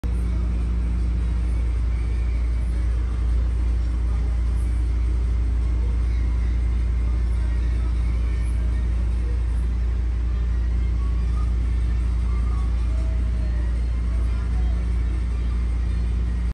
Sound digital weather proof power sports amplifiers coming soon 🤫
One of the smallest, most powerful, weather proof power sports amplifiers ever made ??!!?? 🤔😎💪🏼🤫